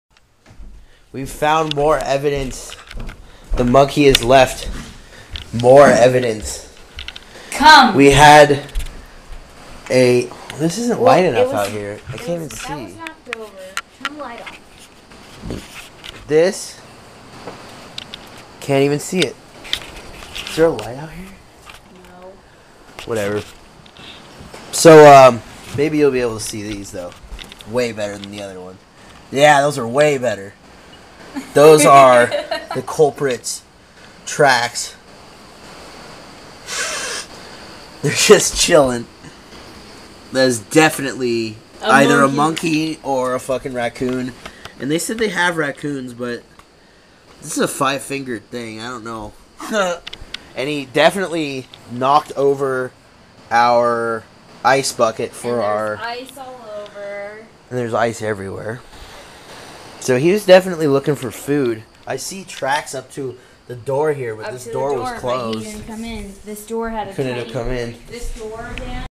Costa Rica Honeymoon Evidence of Monkeys
We found more evidence of whoever stole our dessert plate!